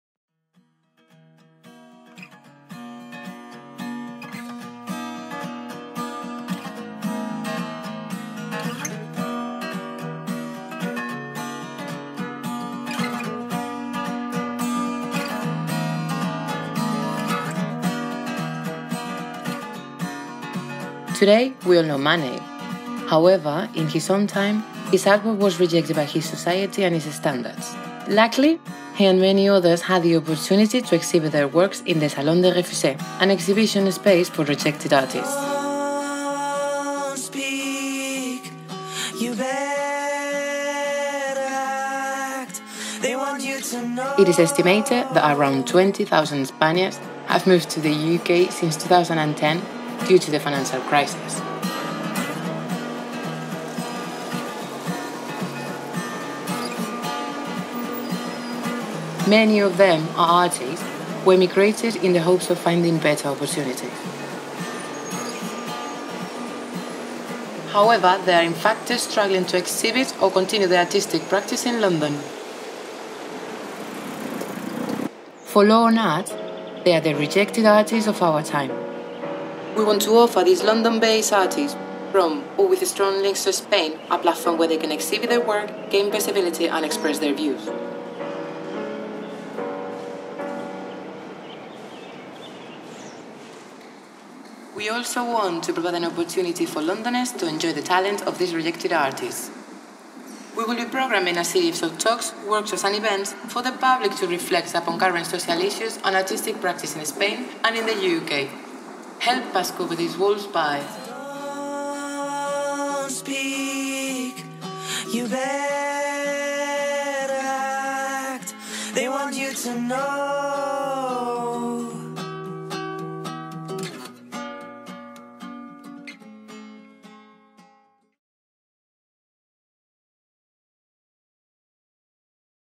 The Salon of Spanish Rejects - Social Exhibition 2014
For our second Social Exhibition, Lon-art.org aims to give a platform to Spanish artists who, because of the economic crisis, have had to emigrate to London. Once they  arrived, many find they have to take on unskilled labour in order to fund their artistic practice.

The Salon of Spanish Rejects has thus been conceived with a strong critical spirit, as an event of social protest in which we will denounce the huge chasm of creativity involuntarily left in Spain by these young artists.

As the title of the project suggests, we want to draw a parallel between these Spanish artists, who have been rejected by their country's institutions, and those French artists whose work was exhibited in the famous salons des refusés in 19th Century Paris, because they had been rejected by official exhibitions for not respecting the academic painting rules of the time.

 Just like Manet, Cézanne and Pisarro, these young Spanish artists of the 21st Century have a lot to offer a global Europe, especially regarding the creation of new aesthetic styles and subjectivities belonging to a post-capitalist panorama, in need more than ever of imagination to create a new world based on innovative economic, political and social models.

Loyal to its philosophy of educating through the arts, Lon-art.org will complement the exhibition with a series of workshops. Artists and participants will explore issues currently affecting Spanish society as well as being invited to reflect on their relevance to British society and life in London. Such themes will include:

- Changes to abortion laws
- The public health service and the rights to it of the 'aventureros' ('fortune hunters', as Spanish politicians are calling the young émigrés)
- The Gag Law in Spain, which limits the freedom of expression
- Housing evictions
- Education and cuts in the arts, philosophy and culture
- Corruption